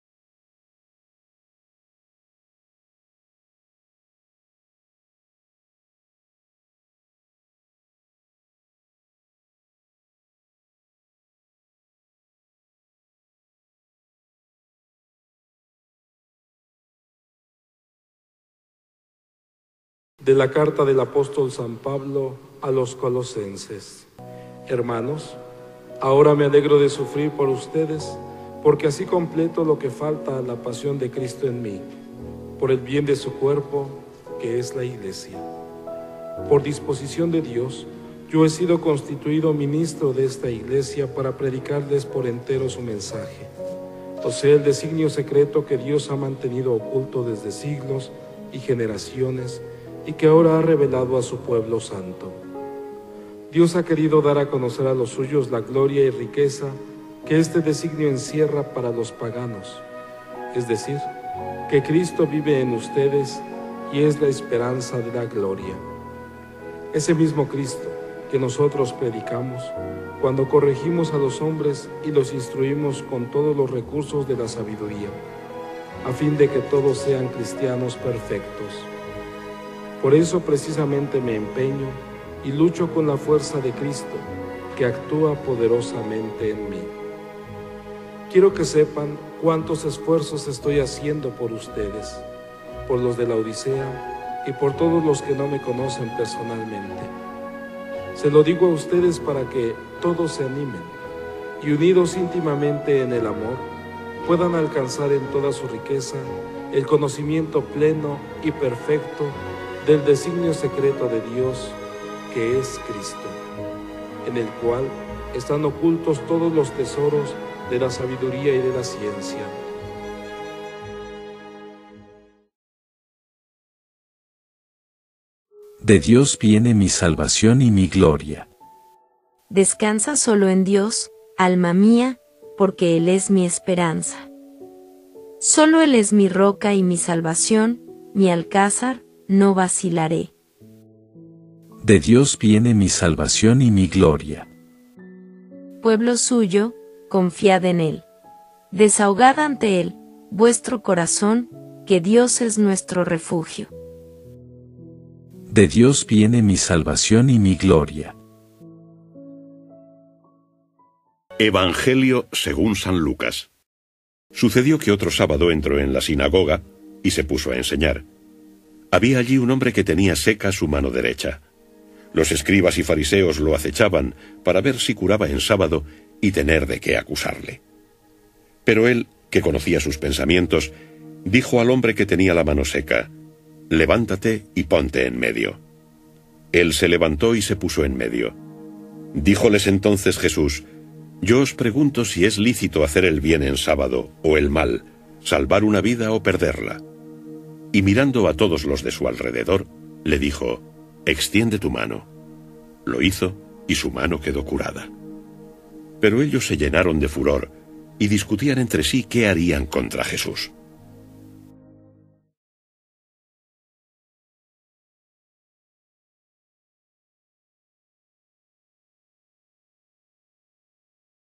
LUNES SEPTIEMBRE 6 DE 2021 | LUCAS 6,6-11
LECTURAS Y EVANGELIO EUCARISTÍA LUNES SEPTIEMBRE 6 DE 2021 LUNES XXIII SEMANA DEL TIEMPO ORDINARIO CICLO B
Primera Lectura: De la carta del apóstol san Pablo a los Colosenses Col 1,24–2,3 Salmo 61,6-7.9 R/. De Dios viene mi salvación y mi gloria Evangelio según san Lucas 6,6-11